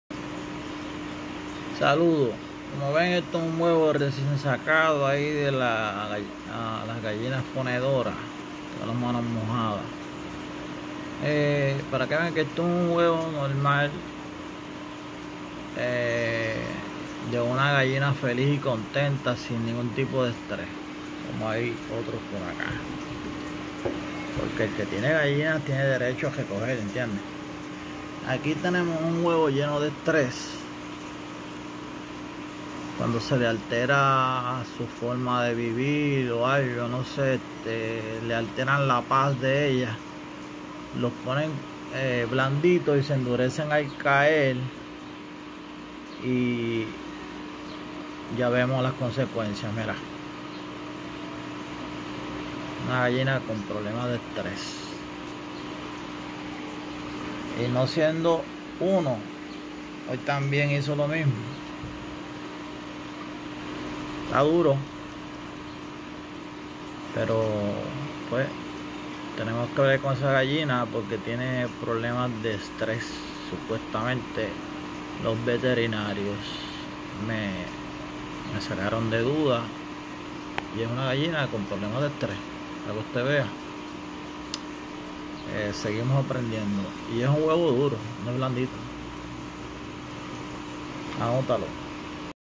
cuando las gallinas sufren estrés
el estrés en las gallinas se refleja en la producción de los huevos...

aquí un ejemplo de una gallina joven.